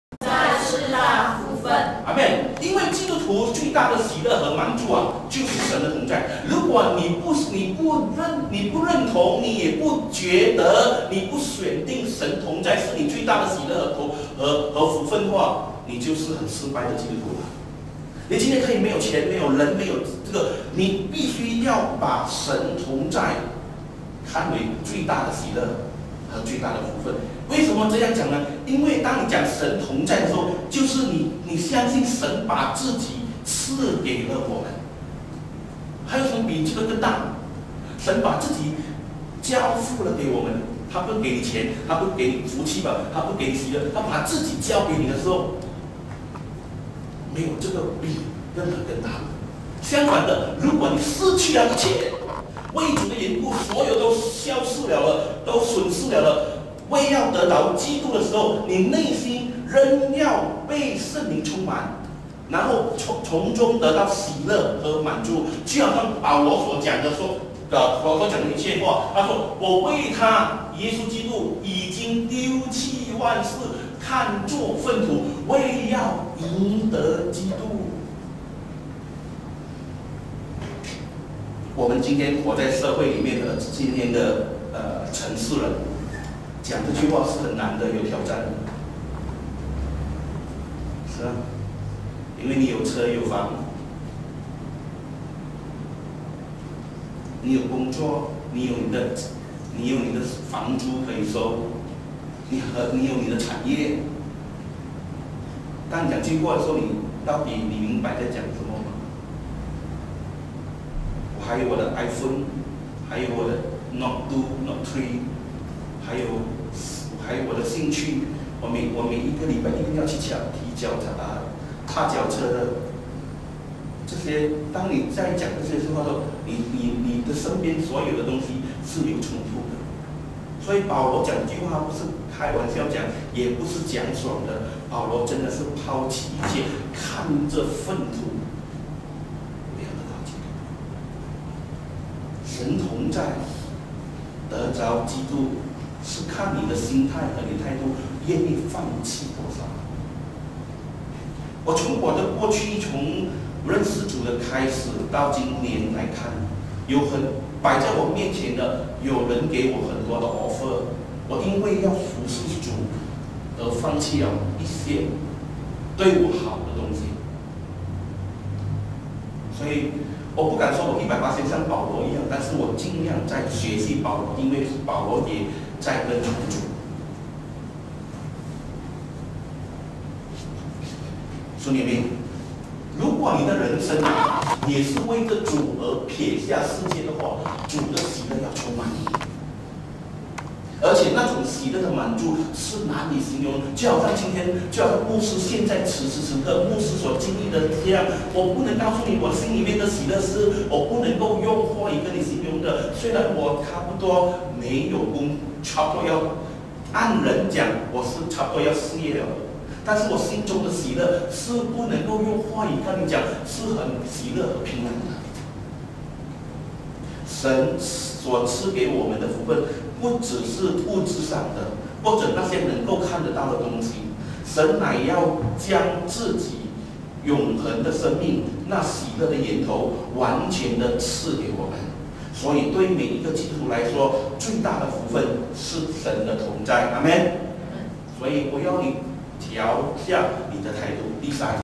神同在是大福分
神同在系列完结篇 (2.2)
《神不在的同在》讲道精华
曾仪生牧师证道
2014年02月09日，星期天